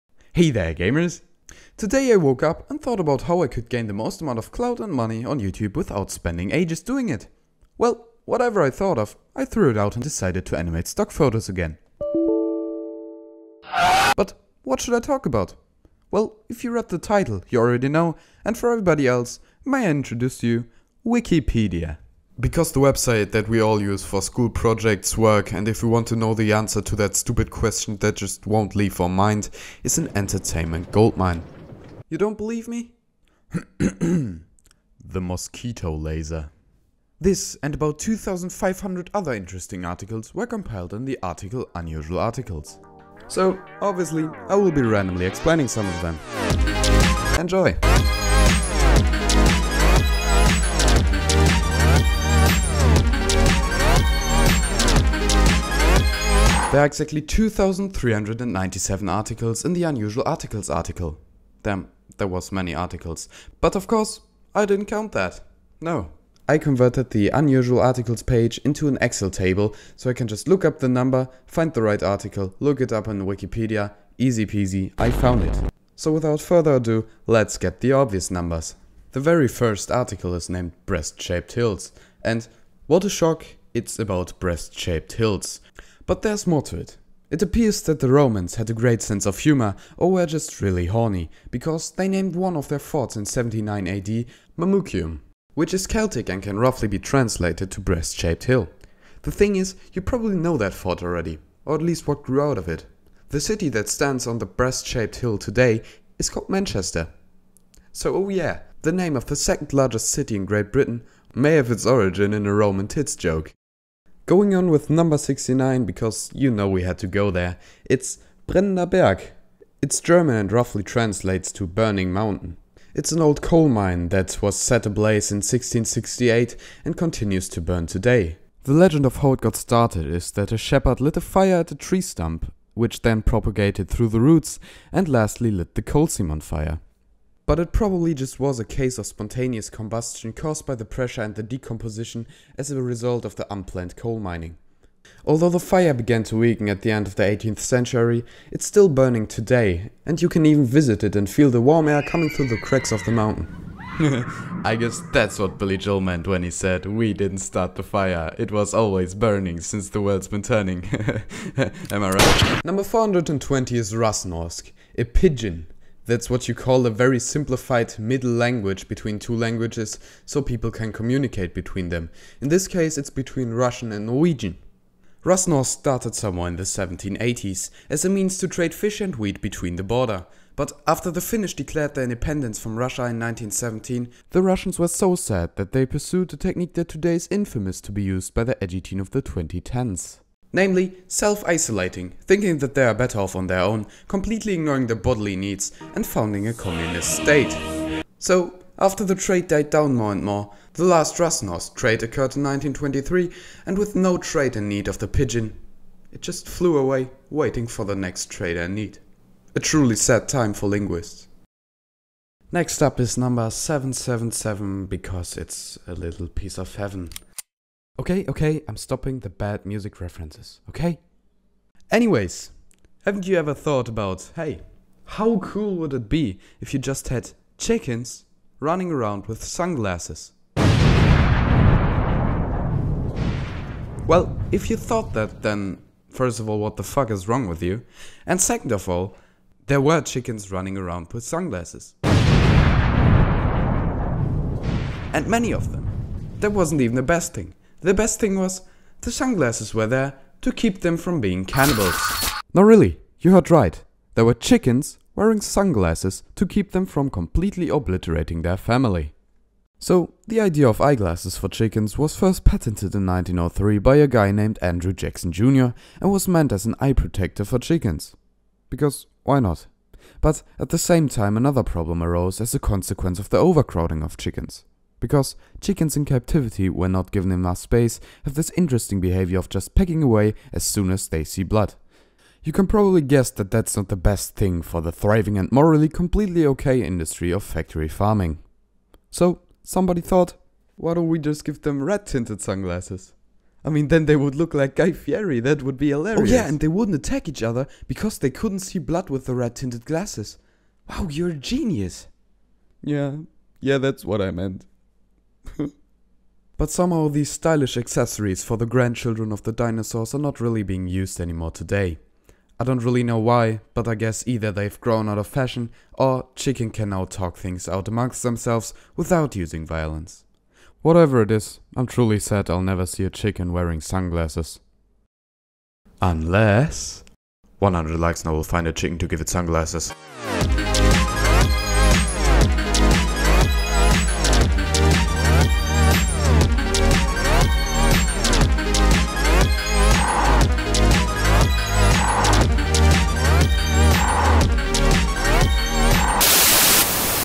Explaining Unusual Wikipedia Articles
Explaining random Wikipedia articles from the article "unusual articles"

(What did you expect?)

Timecodes:
0:00 - Intro
0:55 - How It works
1:19 - Breast-shaped Hills
1:58 - Burning Mountain
2:43 - Russenorsk
3:33 - Chicken glasses
5:30 - Outro